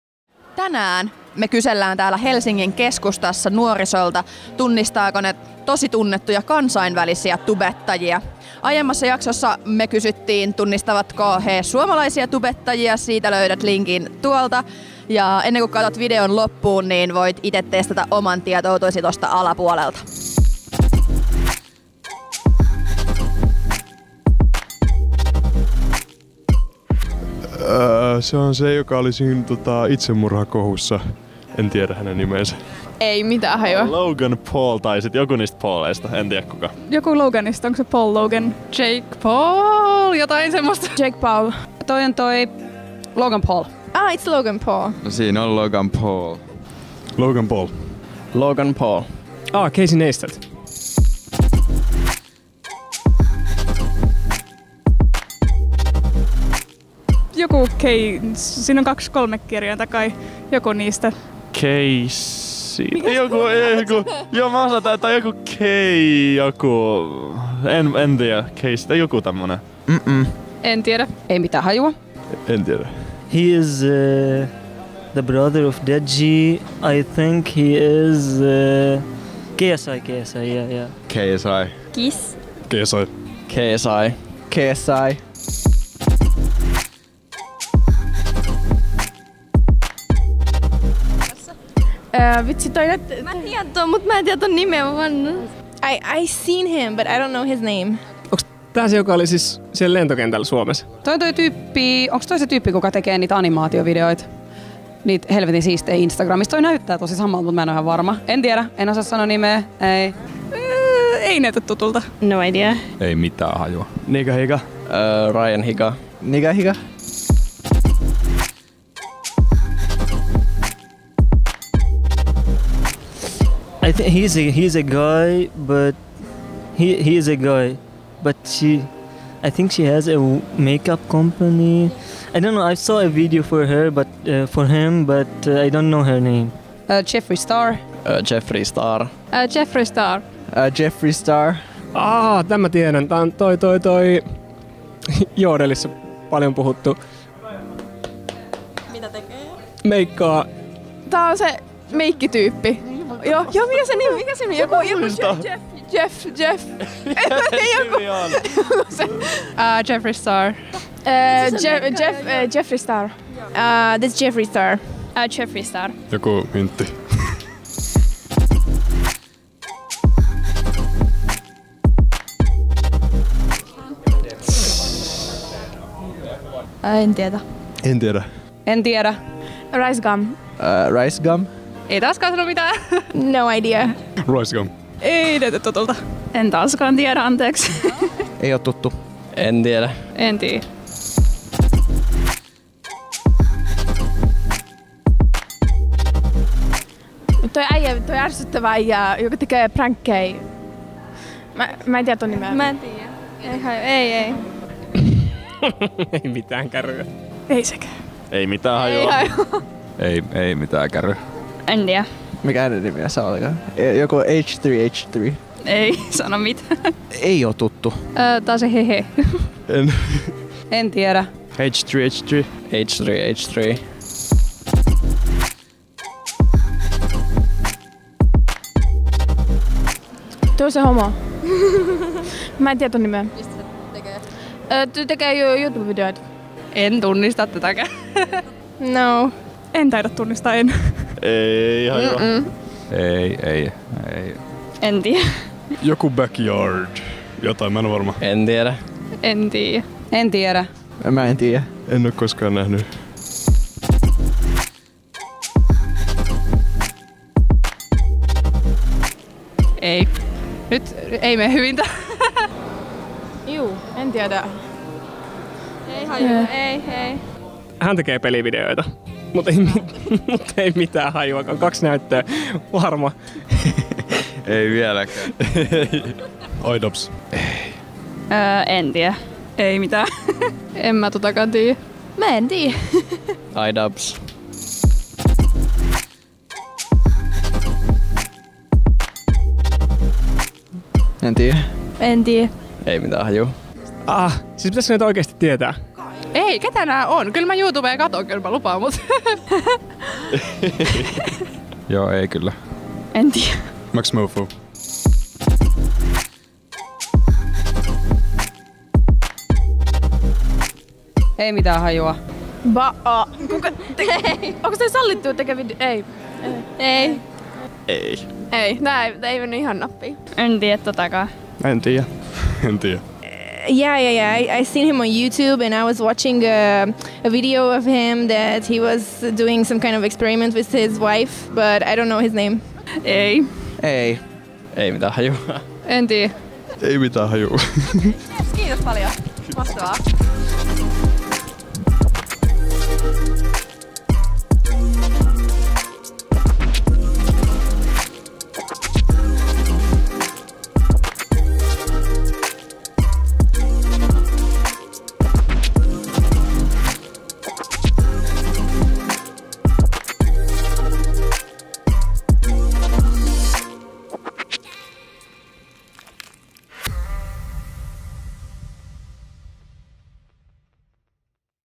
Tunnistaako nuoriso Youtuben supertähdet
HOXHOX! Ennenkuin katsot jakson, käy itse kokeilemassa tunnistatko kaikki tubettajat

Niille jonneille, jotka sekoittaa termin "nuoriso" termiin "lapsi" tai "teini", tässä lainaus Suomen Nuorisolaista:

3 §
Määritelmät
Tässä laissa tarkoitetaan:
1) nuorilla alle 29-vuotiaita;

Joten turpa kiinni penskat. Meiltä menee noin kaksi sekuntia antaa nimimerkillenne bannit kommenttiosastolla, joten se ei ole kovin suuri vaiva.

Rakkautta!